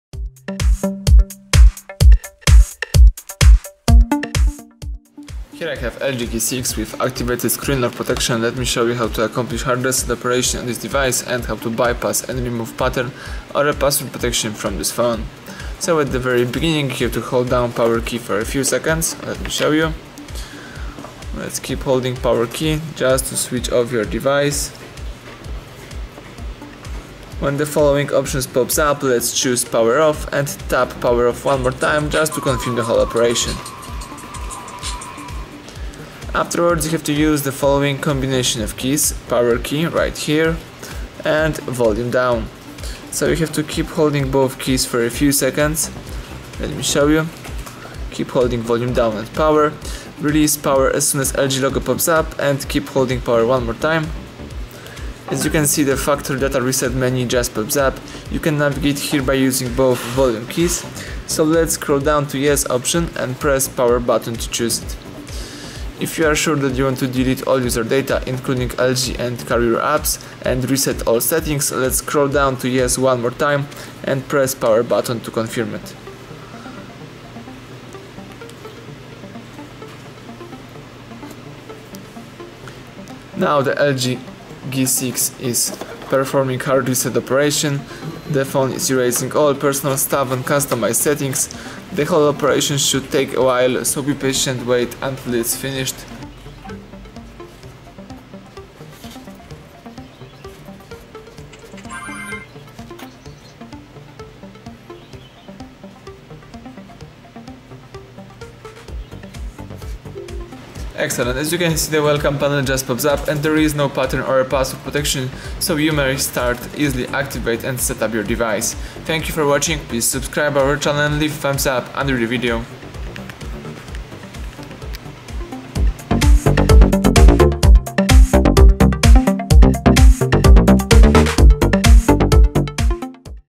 LG G6 HARD RESET / Bypass Screen Lock / Hardware Keys Reset
How to hard reset LG G6? How to factory reset LG G6 ? How to delete all data in LG G6? How bypass pattern protection in LG G6? How clear eMMC in LG G6?

Enter the Factory Mode. Then select wipe data / factory reset. After taht your smartphone will be erasing all personal data and customized settings. After LG Welcome panel pops up you will be able to set up your phone as new.